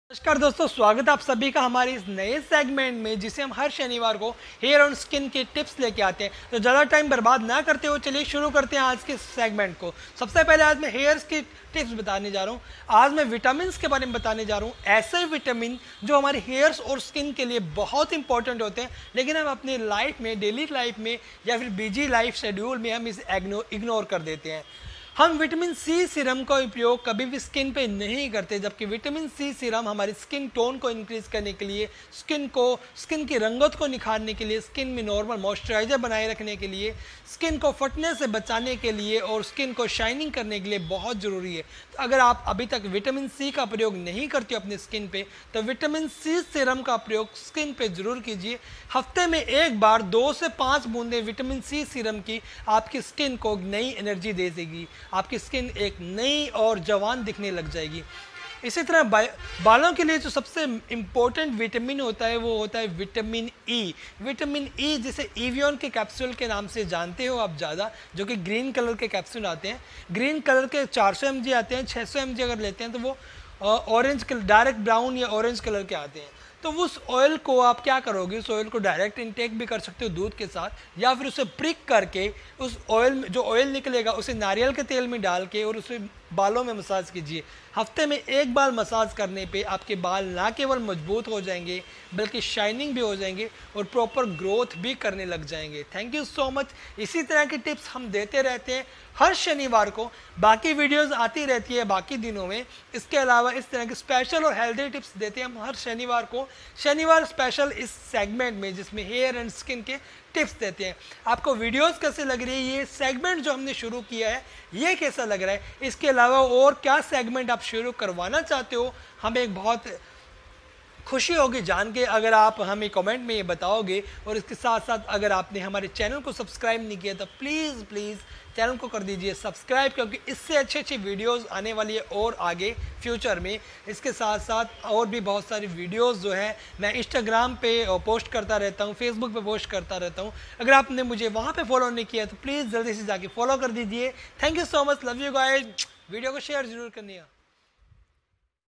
बालो की ग्रोथ और स्किन को glowing बनाने के घरेलू उपाय | Hair and Skin Tips on youtube
hello friend is youtube video me main aap sbhi ko btaunga ki
aap kese hair our skin ko ghrelu upay ke dwara healthy bna sktey ho
hair ke tips me mene hairs ke serum and vitamin e jo ki evion eke nnam se milta h uske bare me btaya h
is se aapke hair fall and hair loss band ko jayega, new hair regrow honge
skin ke segment me mene ke liye importent vitamin vitmin c ke bare me btay=a h
is ghar pr skin care ke roop me use karne se aapki skin gori, khubsurat our glowing ho jayegi
skin care and hair care ke ye ghrelu tips, ghrelu upay aapko bhut fayda denge
thanks